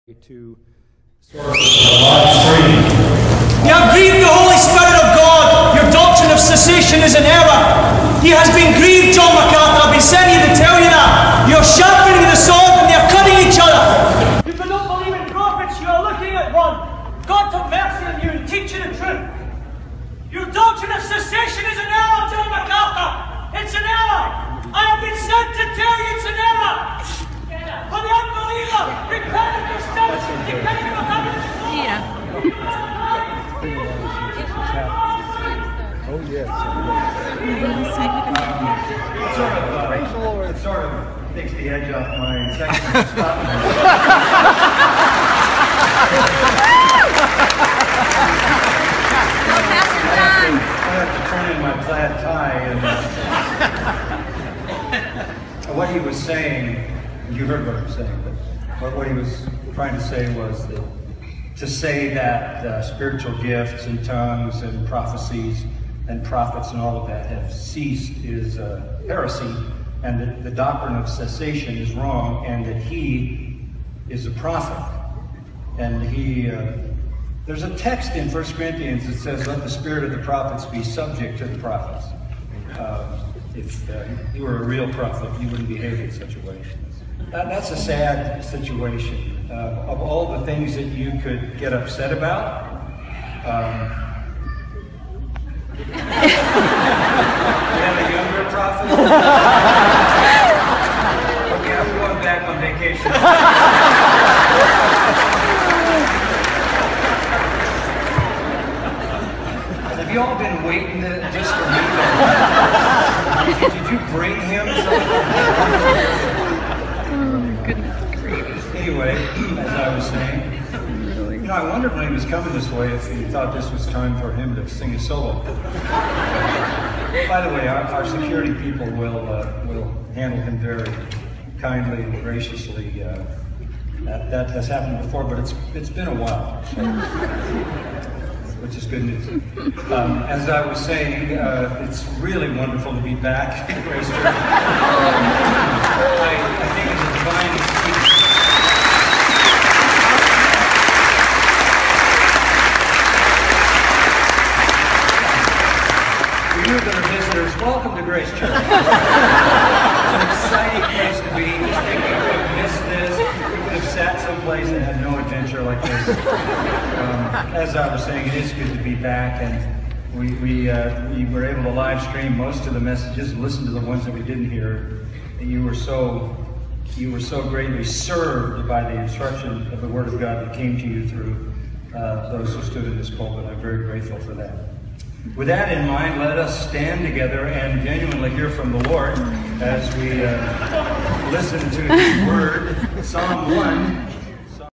Ultimo llamado al arrepentimiento de parte de Dios a John MacArthur antes de mandarle juicio
Dios manda un profeta a John Macarthur para que se arrepienta.
El y su congregacion se burlan de el.

Credito al que originalmente le puso los subtitulos a este video.